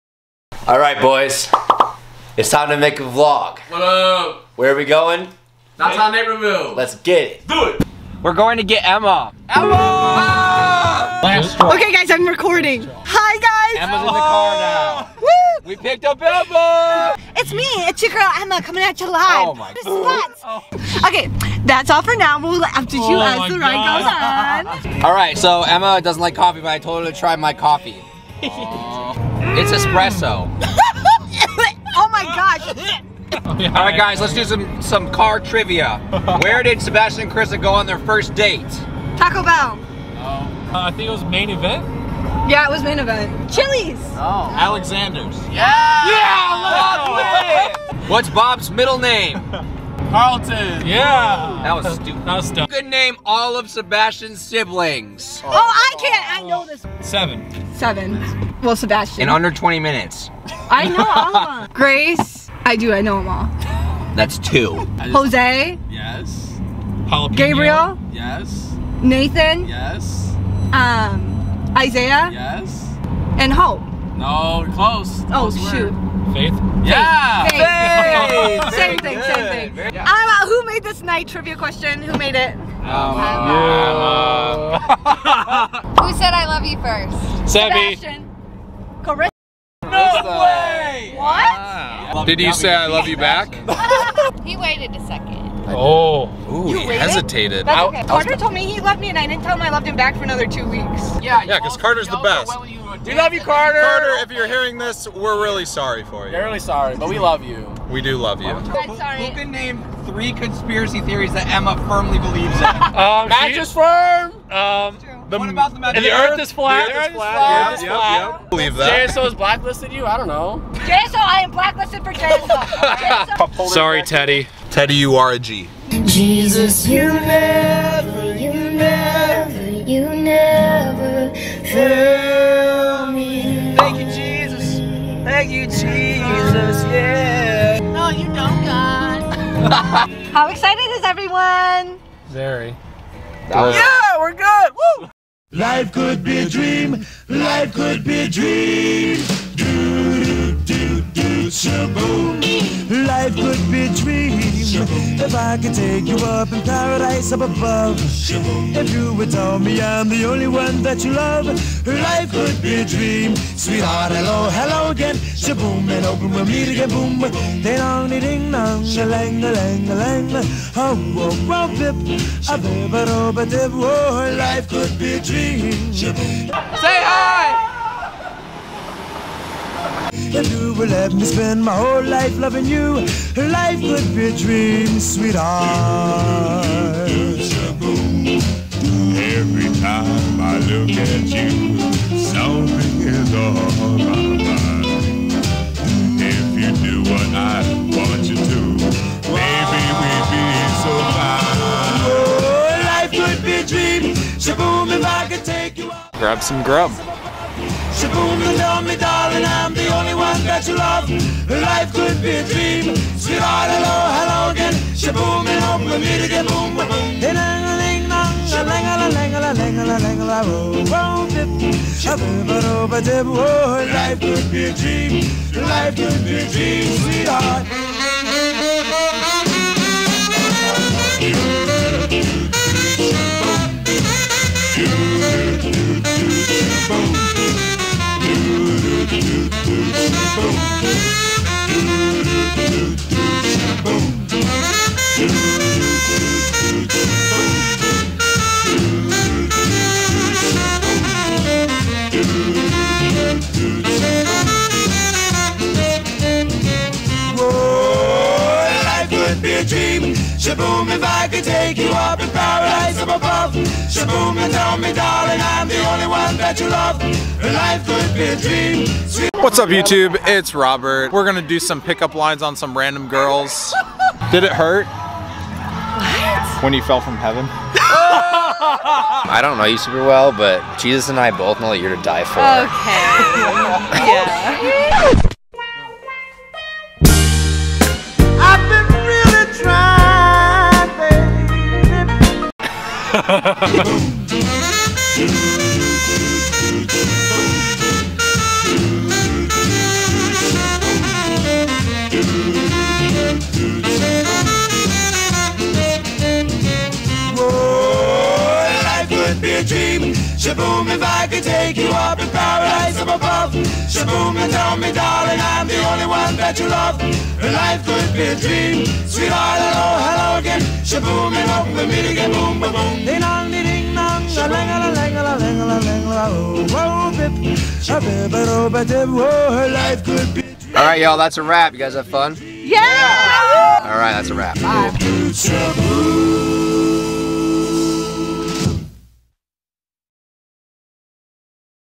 LPS Vlogs: a Night in Naperville
Some friends and I went out on the town.  We vlogged it. Hope you enjoy!
Location: Naperville, IL